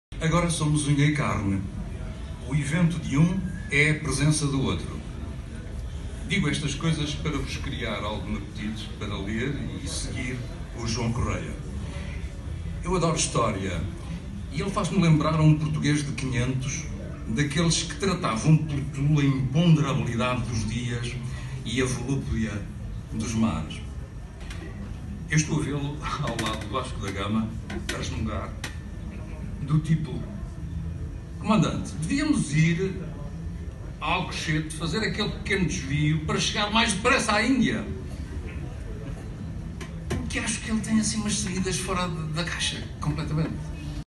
Lançamento do "Tubarões Voadores" na FNAC Colombo :: 2019-03-19
O evento, que decorreu na FNAC Colombo, contou com a apresentação de Eduardo Boavida (Bertrand Editora), moderação da jornalista Ana Daniela Soares e comentários dos estimados Eduardo Rêgo (Loving the Planet), Miguel Gonçalves (Spark Agency) e David Lopes (AEON Topvalu Co.)

As imagens foram captadas pelo Diogo Almeida Alves.